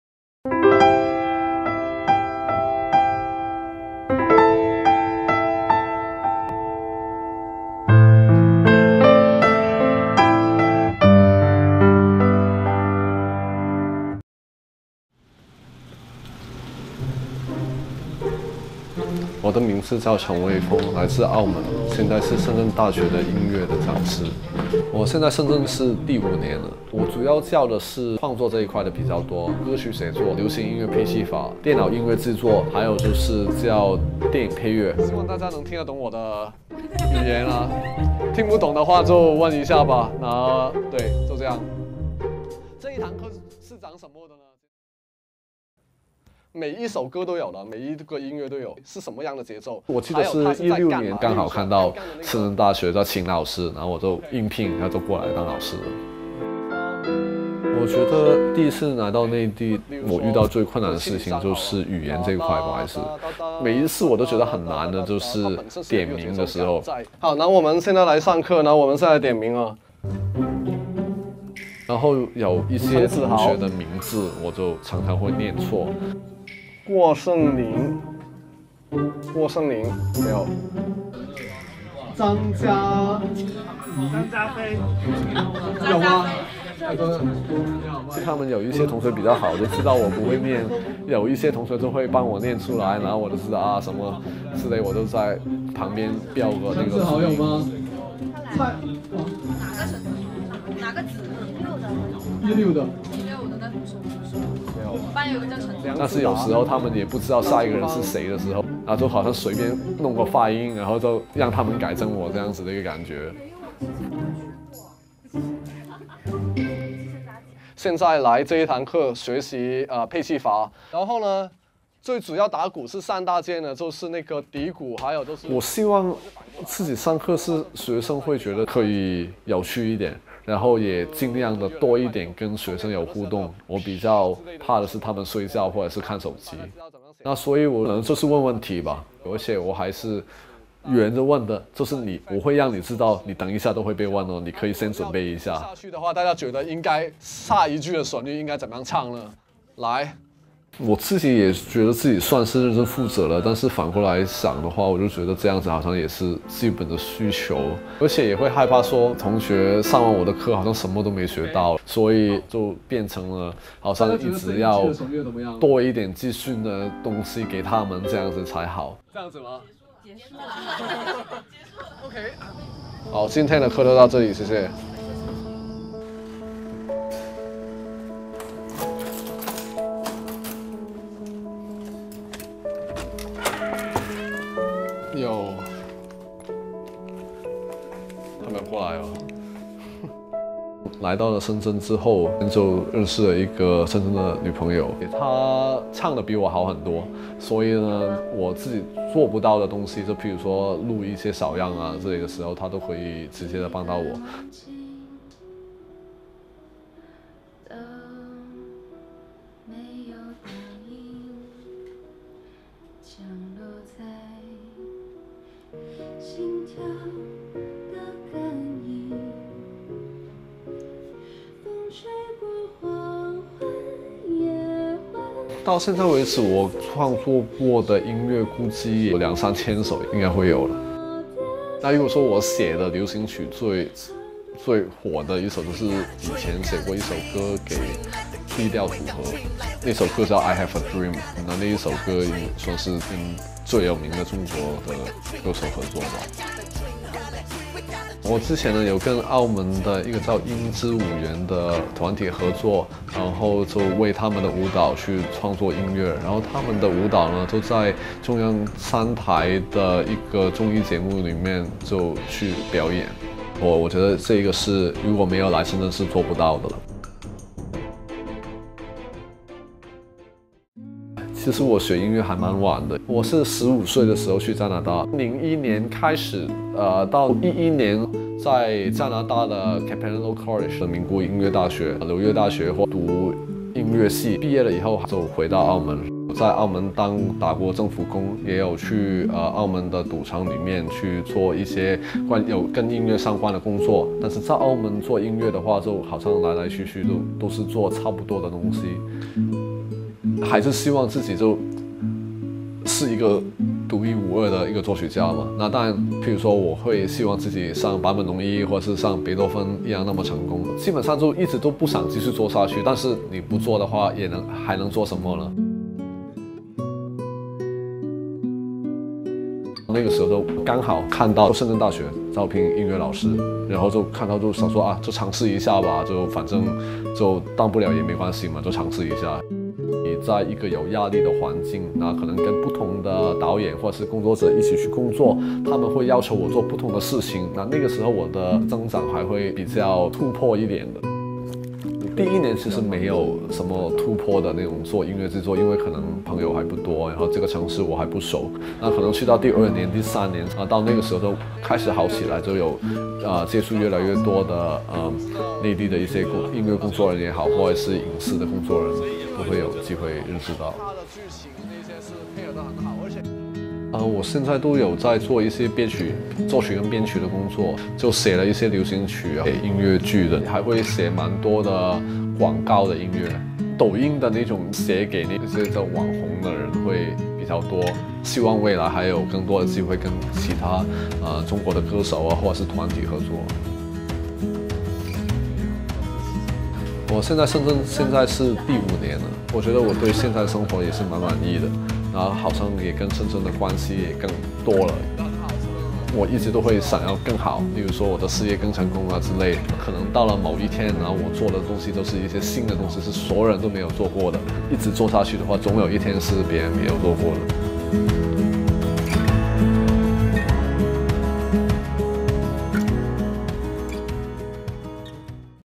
澳门青年深圳大学教音乐，事业有发展，爱情有收获
在这个热衷讨论梦想的时代，澳门音乐人陈蔚风给出了自己的答案。陈蔚风年轻时曾经在加拿大、日本、美国等国深造音乐，回国后在澳门做过各种工作。怀揣音乐梦想的他，最终选择来到深圳大学做一名音乐讲师，并开启了音乐创作的新征途。